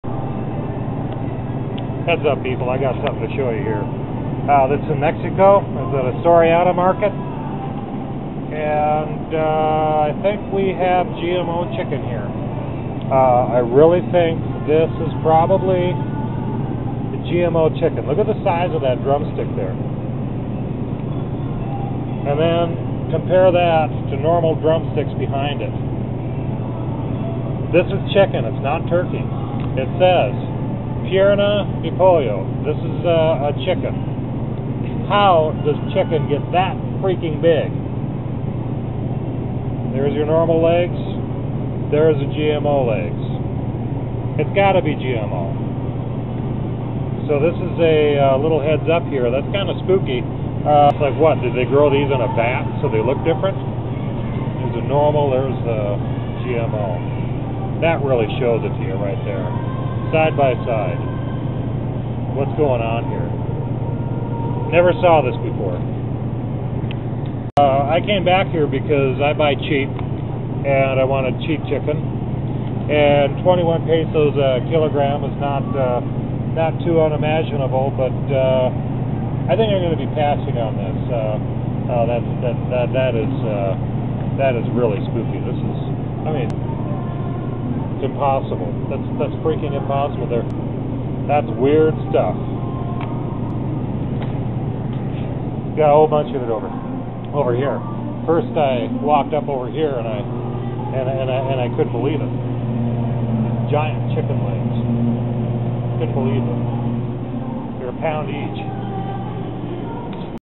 gmo food chicken not so little THUMBNAIL BLOCKED, Video too much for Youtube?
GMO CHICKEN, I SHOT A VIDEO OF IT IN THE STORE YESTERDAY AND IT IS NOW CONFIRMED BY THE LA TIMES ON MAY 3 2015 THAT THOUSANDS OF SPECIES HAVE ALREADY BEEN MODIFIED.  THEY ARE OBVIOUSLY BEGINNING THE MARKET ROLLOUTS
"CRISPR/Cas9 makes it possible for nearly any scientist to edit DNA in nearly any cell. In the last couple of years, scientists have used it to edit genes in adult human cells"

BLOCKBUSTER UPDATE: THEY WERE NOT BEING HONEST, THEY JUST ADMITTED VIA THE L.A. TIMES TODAY THAT THOUSANDS OF LABS HAVE ALREADY MADE PERMANENT TRANS GENERATIONAL GENETIC MODIFICATIONS TO THOUSANDS OF DIFFERENT SPECIES AND THEREFORE THE CHICKEN IN THIS VIDEO PROBABLY REALLY IS GMO!
In a report released by the LA times today (Sunday, May 3, 2015) it has been admitted that Scientists have already made genetic changes to thousands of organisms, and that a new technology had been developed that allowed common scientists to design and implement changes to DNA of ANY ORGANISM that are permanent and passed along through the generations.

OBVIOUSLY THE CHICKEN IN THIS VIDEO I SHOT YESTERDAY REALLY IS GENETICALLY MODIFIED, AS I SUSPECTED.
All they did was keep the fact that they were tweaking practically everything (thousands of species) hush hush until it was way beyond too late for the public to protest. Now that the genie is totally out of the bottle and can never be put back in the bottle, they are informing the public. I had already warned people about the permanently genetically modifying T4 nanobots in earlier reports, Now the LA times is breaking the news to the idiots who would never find a web site like this, and it is probably game over. WARNING: IF YOU DO NOT WANT TO BECOME WHAT AN ELITE GLOBALISTS WANTS YOU TO BECOME, NEVER GET A SHOT EVER AGAIN IN YOUR LIFE, THAT IS HOW THE GENE MODIFYING TECHNOLOGY GETS INTO YOU (this was not stated in their article but I already know after being told by friends in the medical community,) and HOW ELSE could it happen anyway?

No wonder why they are now making it illegal or impossible to say no to vaccines in so many places - They want to convert the world population into their servant species. If this was not the case, they would not be trying to make resistance futile! Got your bug out plan? I think it is time to go with it if under force you HAVE TO get a shot!

I am positive I have shot video of genetically modified chicken in the video - now that the LA times is admitting it has been done thousands of times, I can't possibly see this any other way.